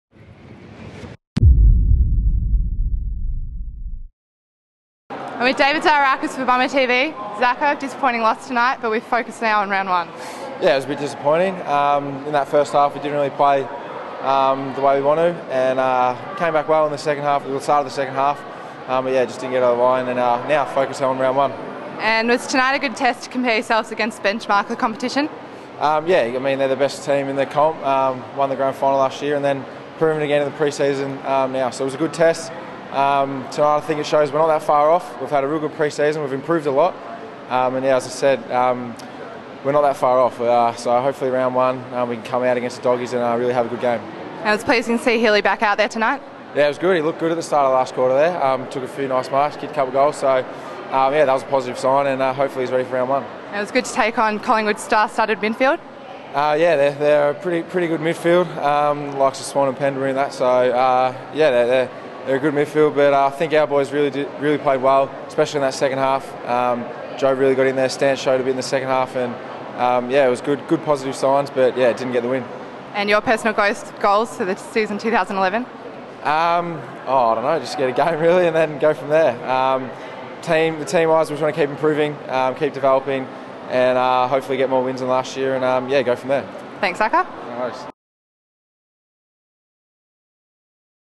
Zaharakis proud of midfield effort
David Zaharakis speaks exclusively to Bomber TV following the NAB Cup Grand Final against Collingwood.